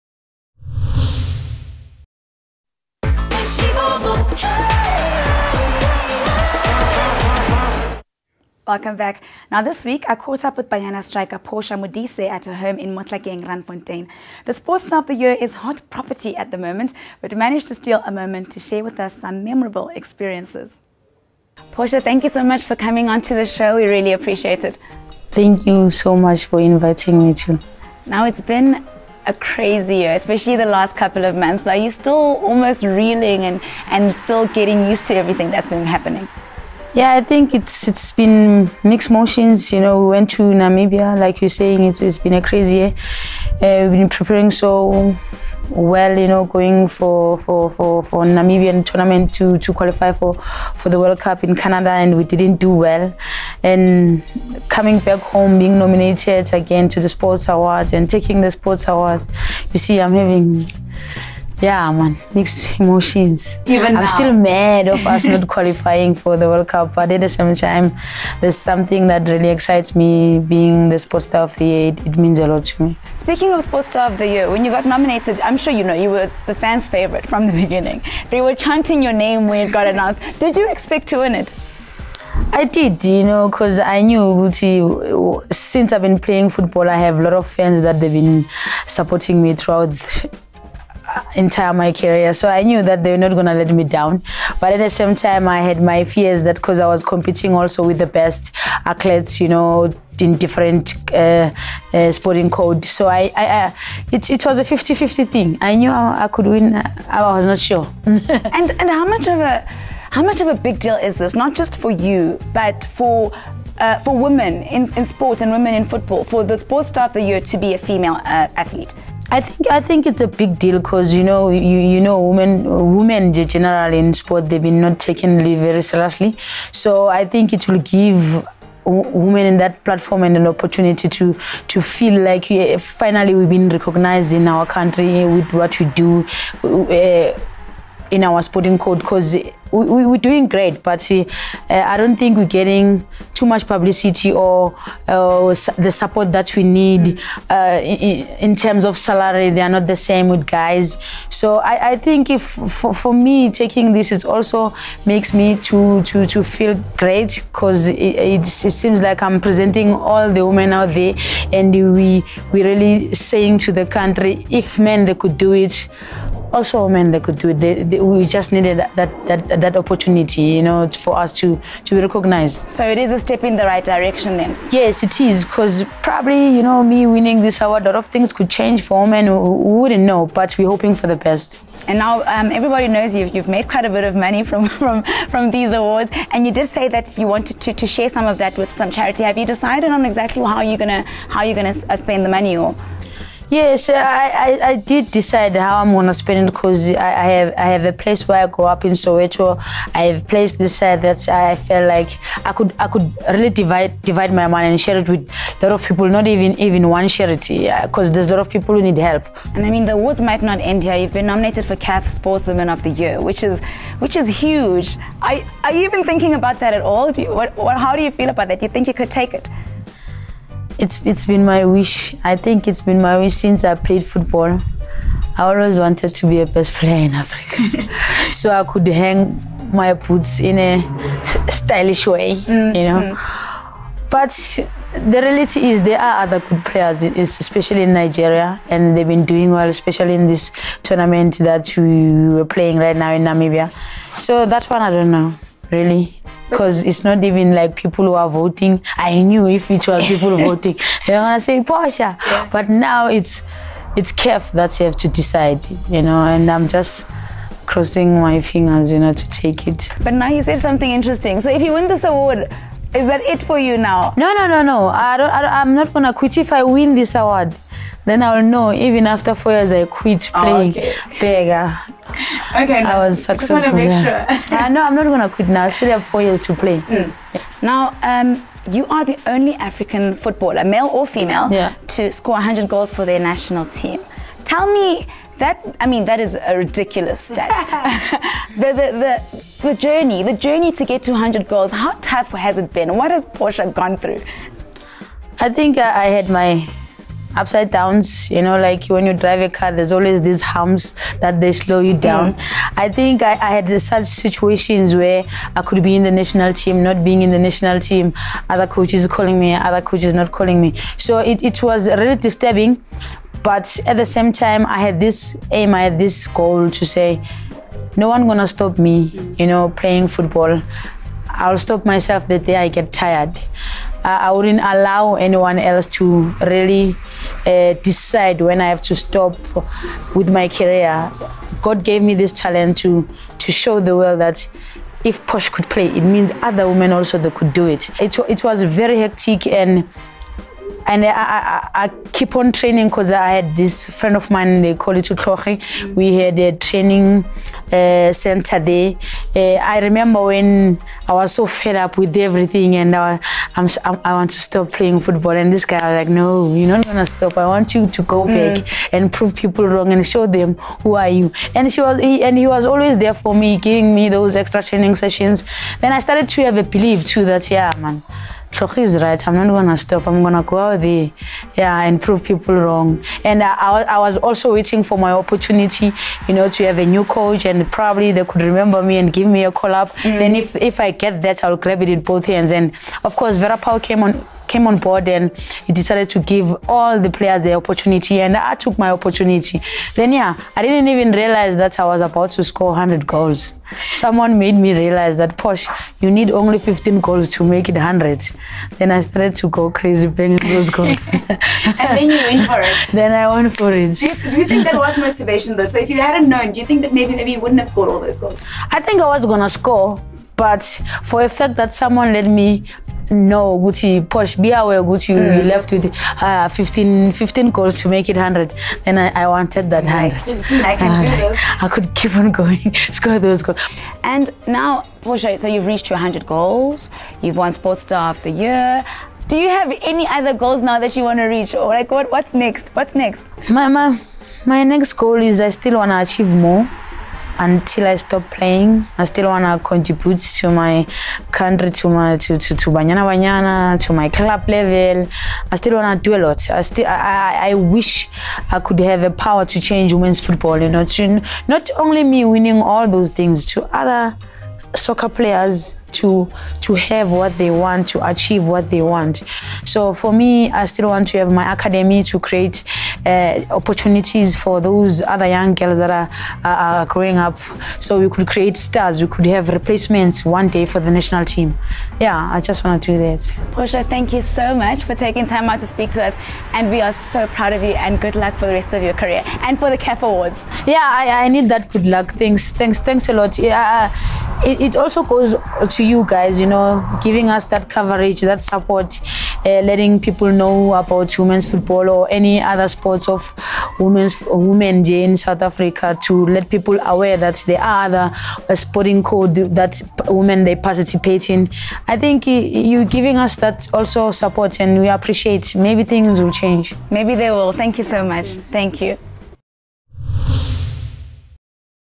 Up close and personal with Portia Modise
eShibobo caught up with Banyana striker Portia Modise at her home in Mohlakeng, Randfontein.